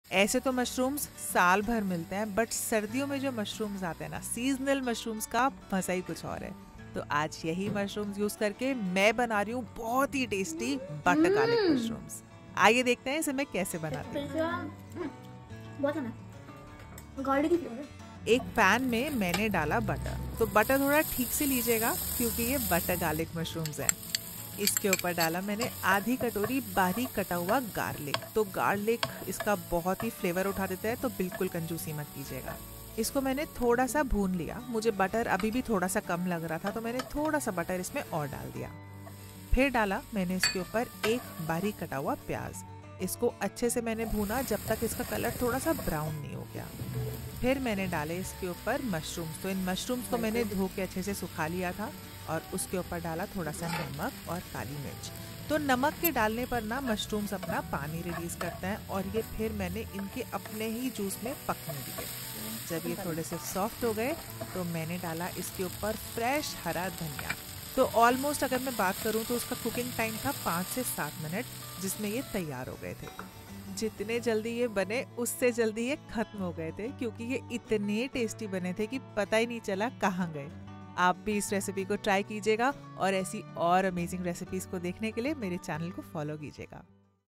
Elevating my kitchen game with these irresistible buttery garlic mushrooms! 🍄✨
Elevating my kitchen game with these irresistible buttery garlic mushrooms! 🍄✨ Sautéed to golden perfection in butter and minced garlic, seasoned with a dash of salt, pepper, and coriander leaves . Simple, yet oh-so-satisfying!